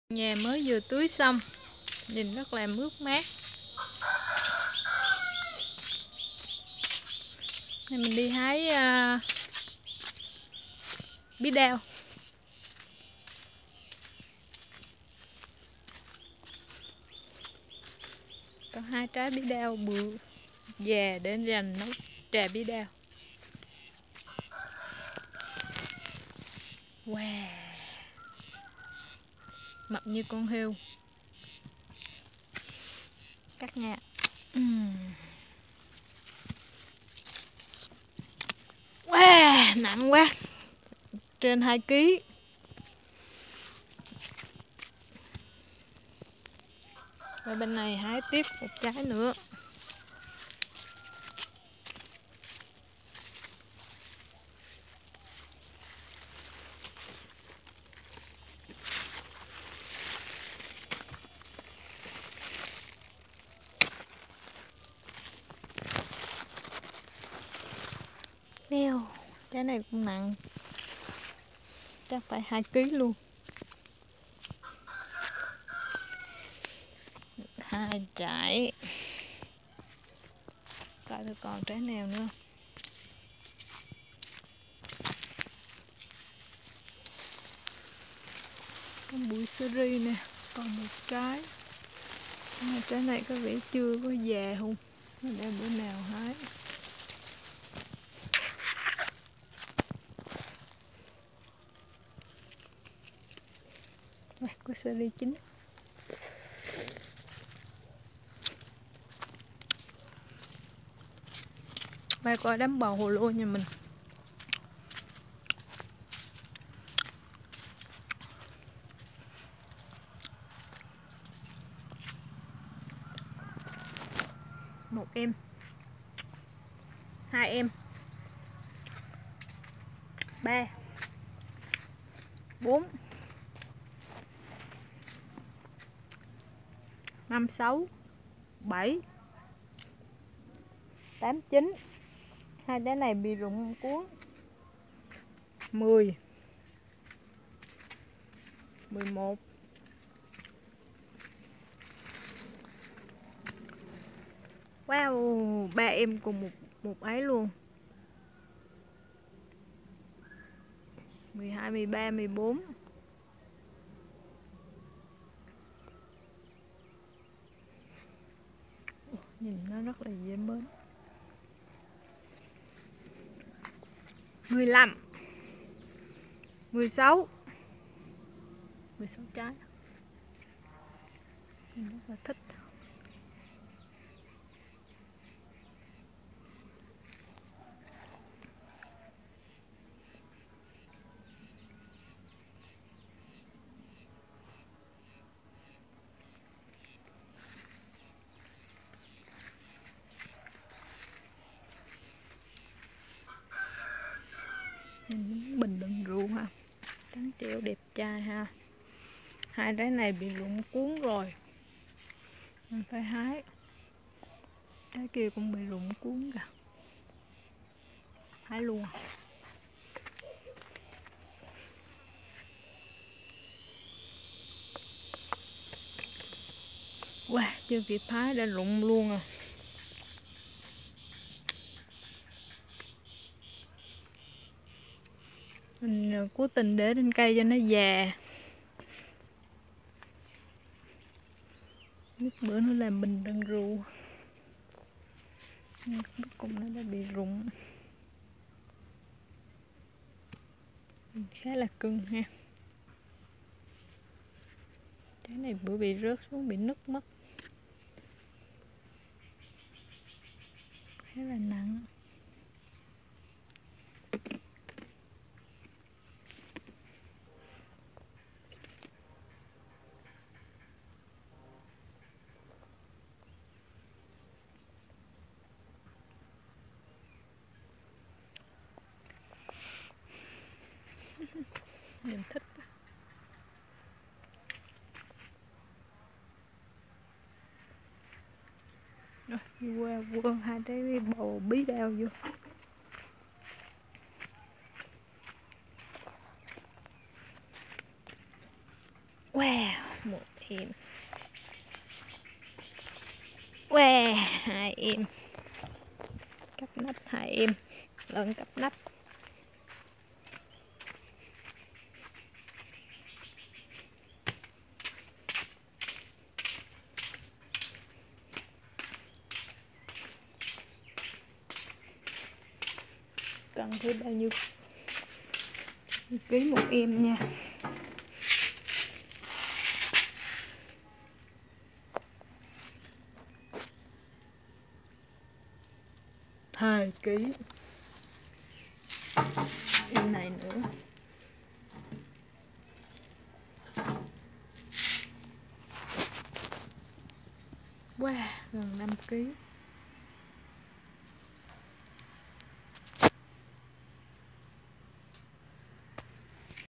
HÁI BÍ ĐAO VÀ BẦU HỒ LÔ | Winter melon and bottle gourd
Chân thành cảm ơn các bạn đã quan tâm và ủng hộ!
© Bản quyền thuộc về Trâm Nguyễn
© Copyright by Tram Nguyen ☞ "Do not Reup"
Cảm giác trồng cây đến ngày thu hoạch thật tuyệt. Những trái bí đao để già thiệt già dành nấu trà bí đao mập tròn như những con heo con lẫn trong đám lá xanh mướt. Những trái bầu hồ lô xanh mướt mát, treo lủng lẳng nhìn rất là dễ thương không kém. Những vất vả khi gieo hạt, nhổ cỏ, tưới nước, bỏ phân, bắt sâu tự nhiên bay biến hết.